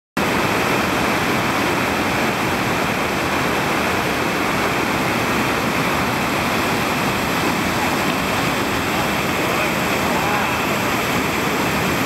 Particle Accelerator(IV), Upper Black River
Open Boater doing Particle Accelerator on the Upper Black.
Nice Line